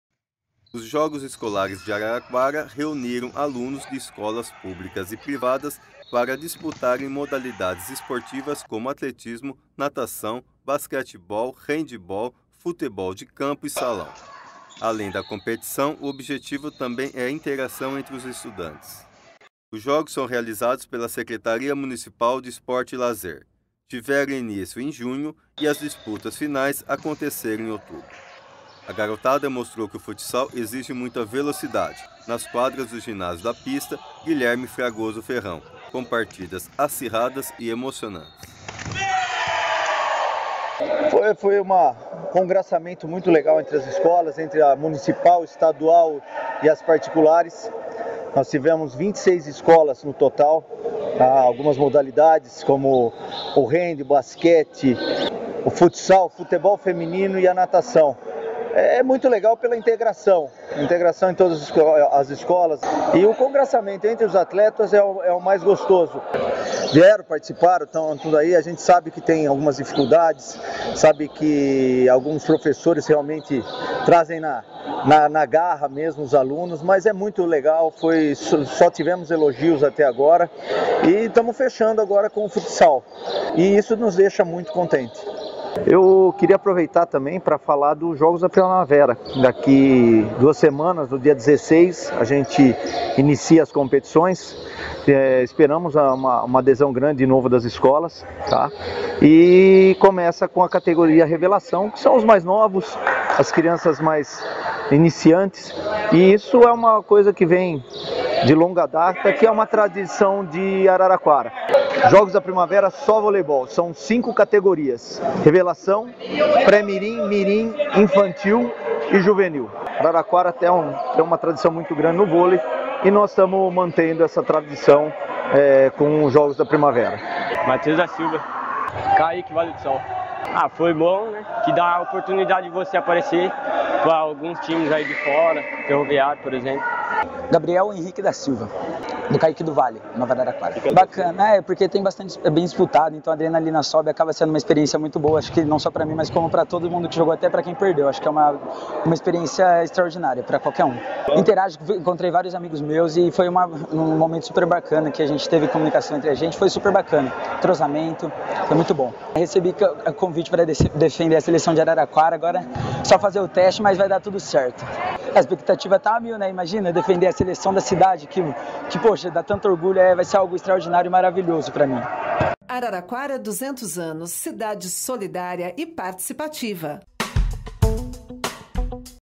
JOCOARA
Jogos Escolares ⚽🏀🏆

A Prefeitura retomou esse ano os Jogos Escolares de Araraquara (Jocoara), com incentivo do prefeito Edinho Silva. Os jogos visam à integração dos estudantes da rede pública e privada, por meio da prática esportiva, além de revelar talentos para as equipes de competições da Fundesport e constar no calendário festivo dos 200 anos de fundação do Município. Confira no vídeo como foi!
Araraquara, 200 anos, com mais esportes!